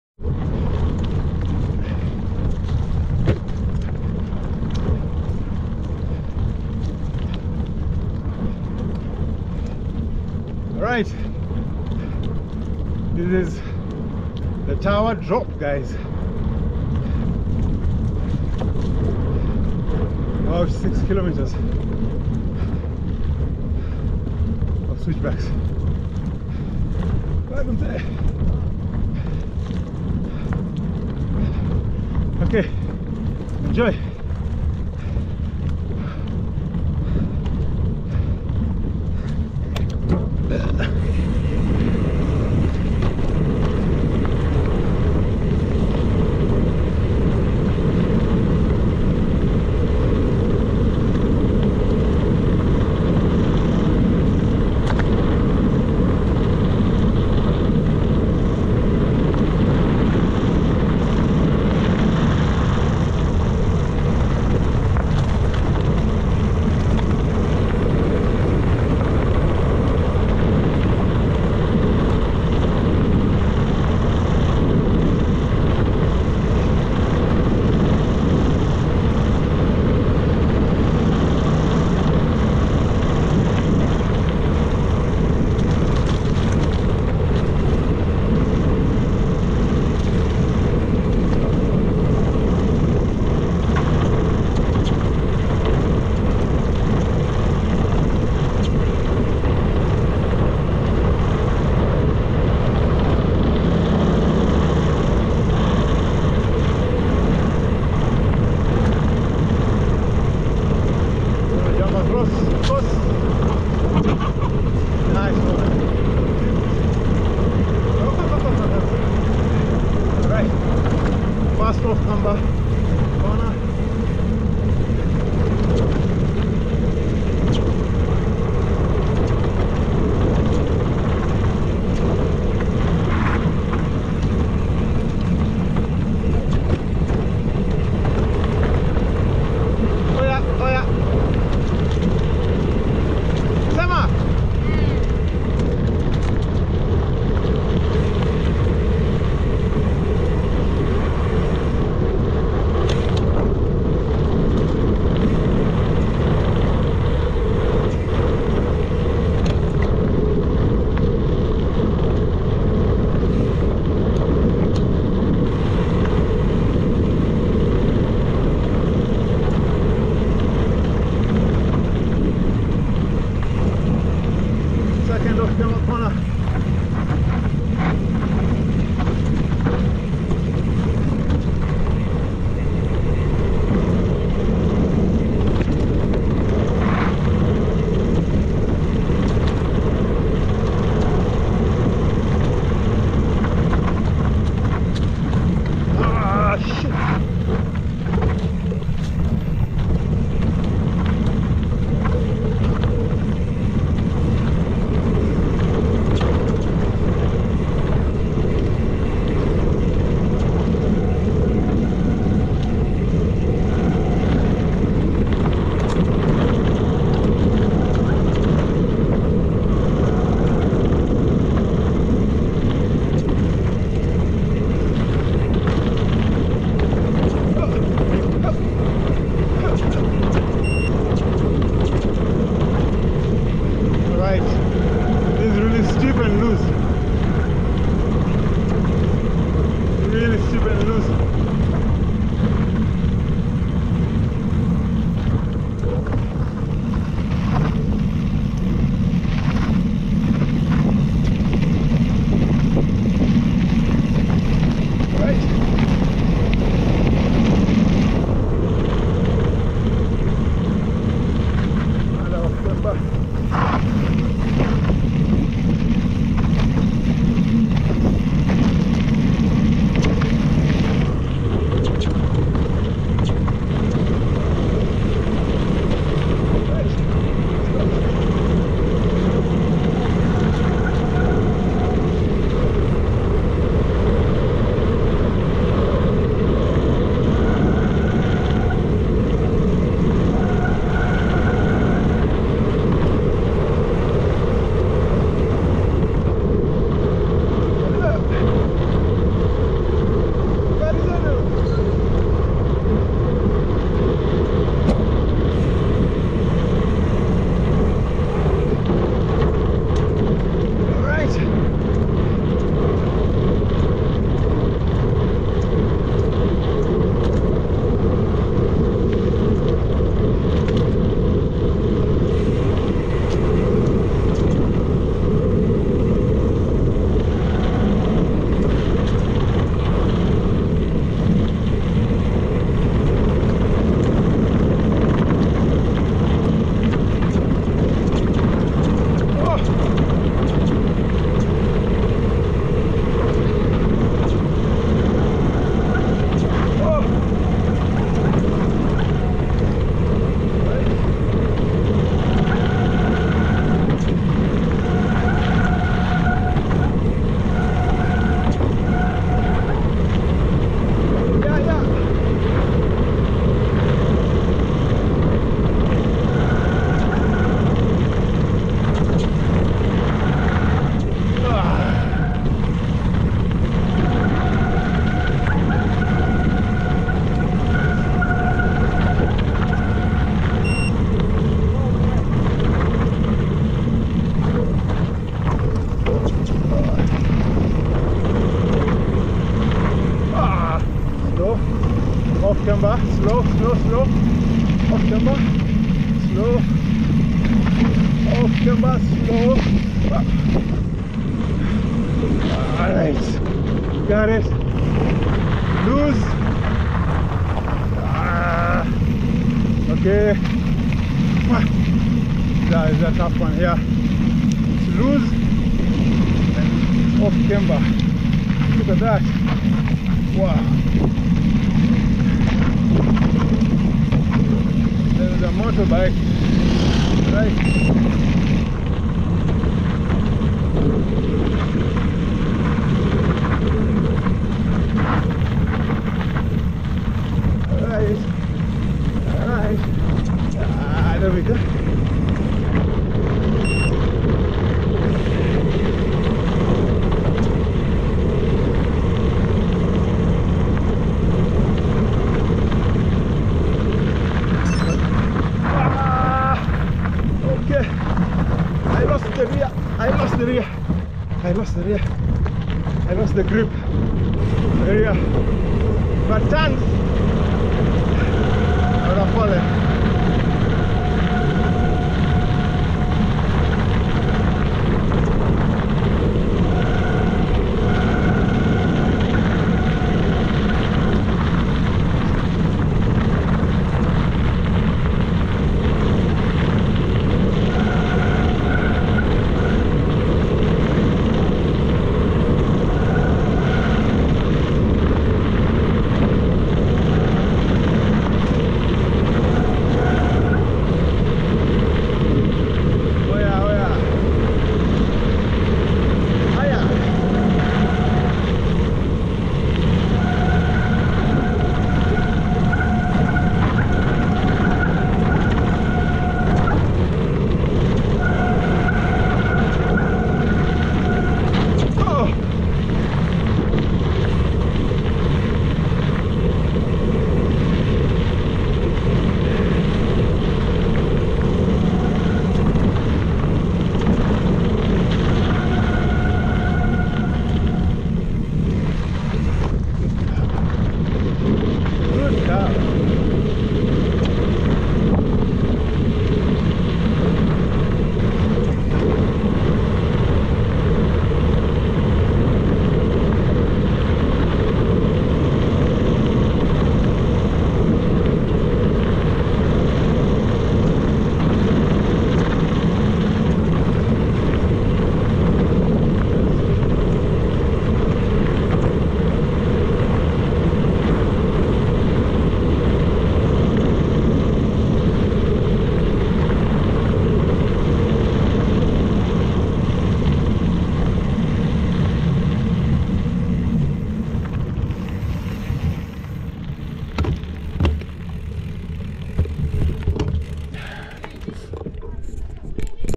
Makueni MTB: Tawa Drop. Up to -50% gradients!
Arrived in Tawa early and decided to do a spin up the iconic Tawa climb. Coming down is always fun!